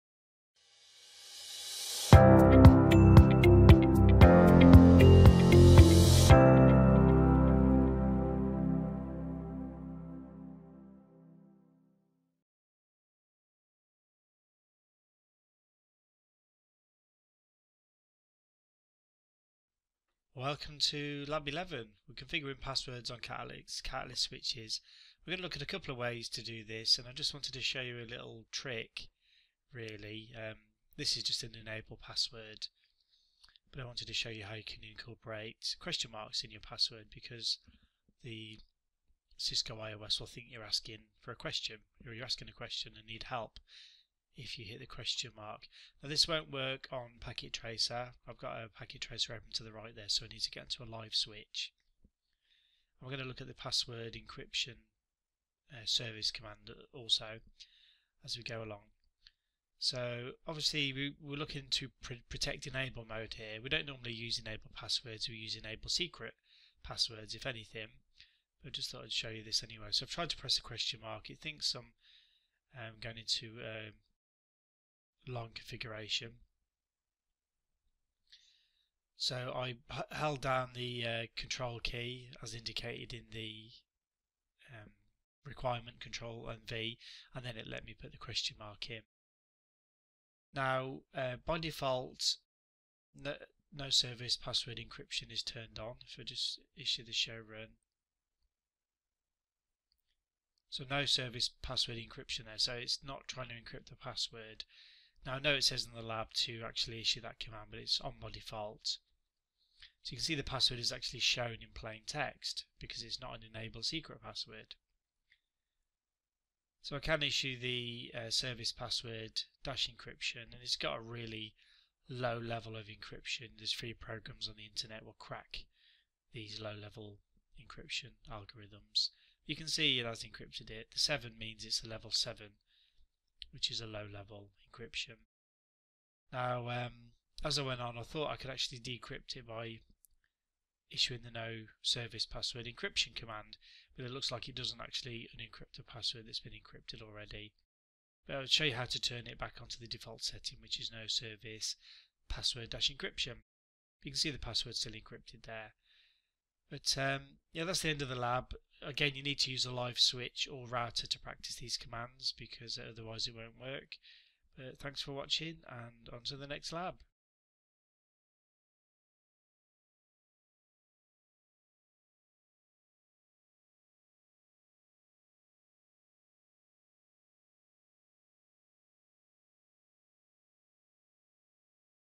Configuring Passwords on Catalyst Switches
- one of our 101 Labs - Cisco CCNA training videos.